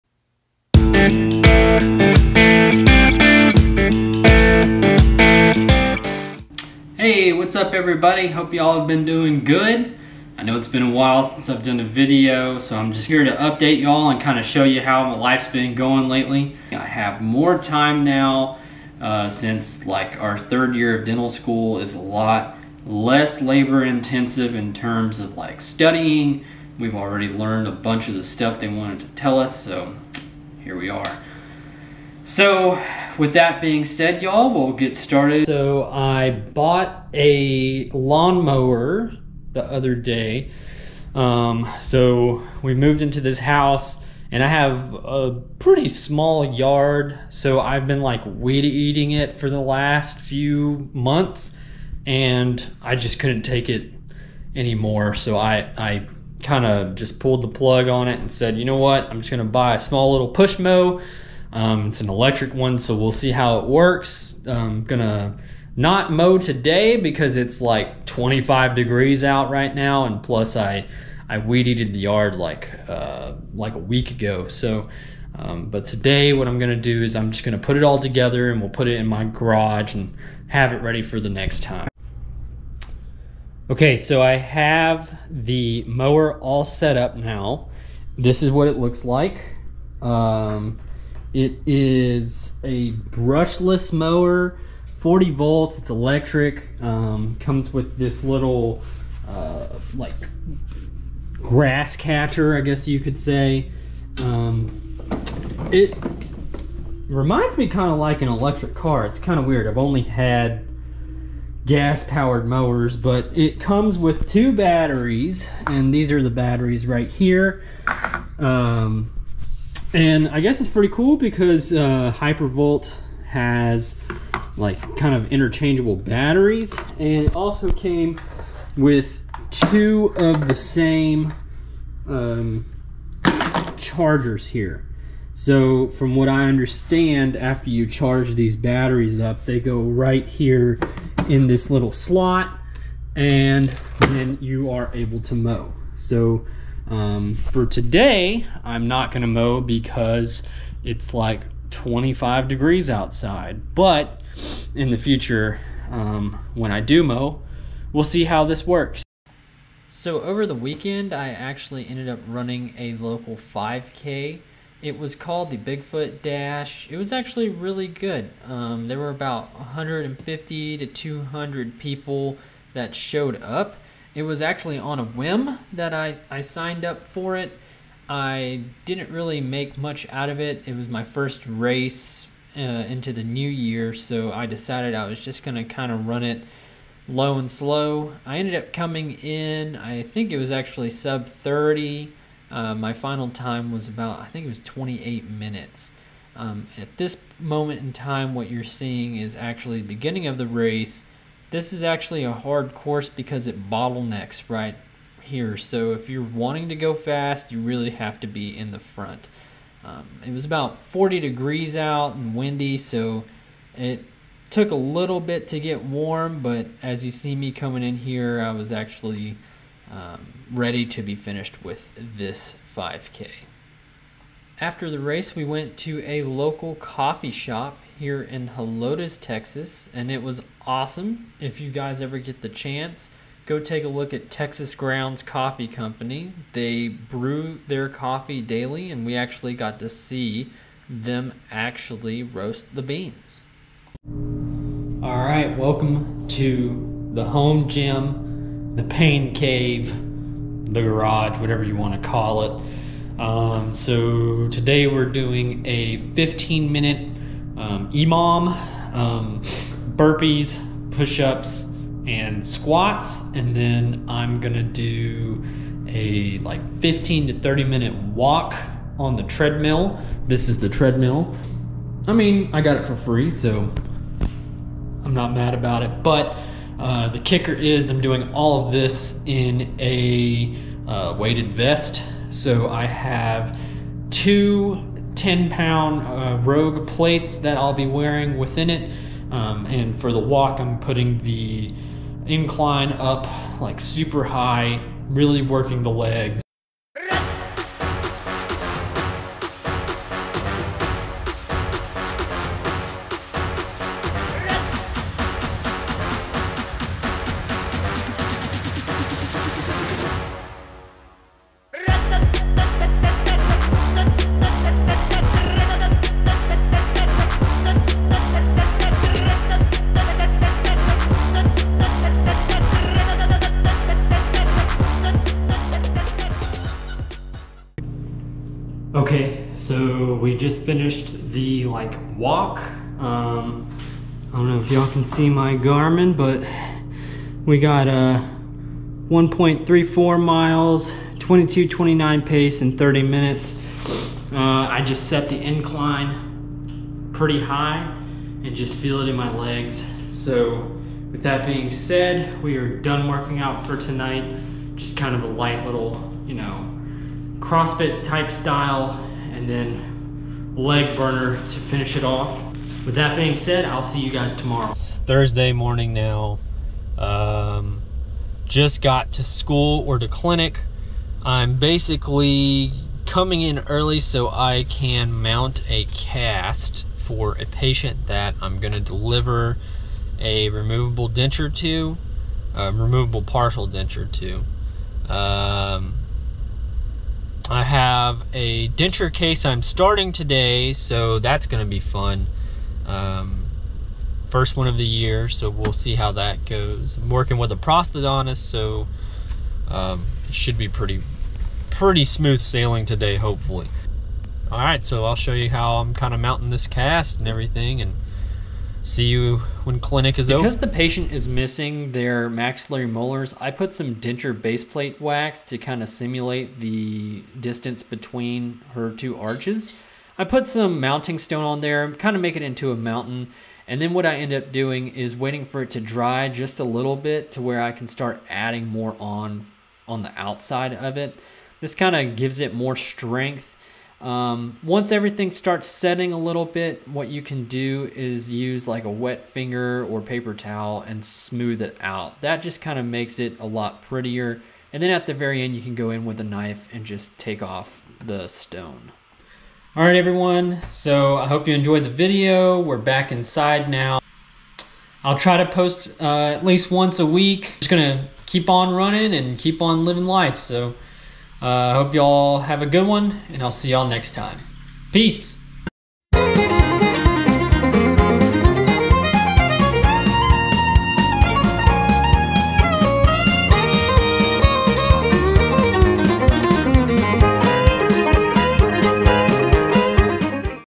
Back to the Grind
In this video you get to see my first run back in January, as well as a new lawn mower and my life as a 3rd year dental student here in Texas!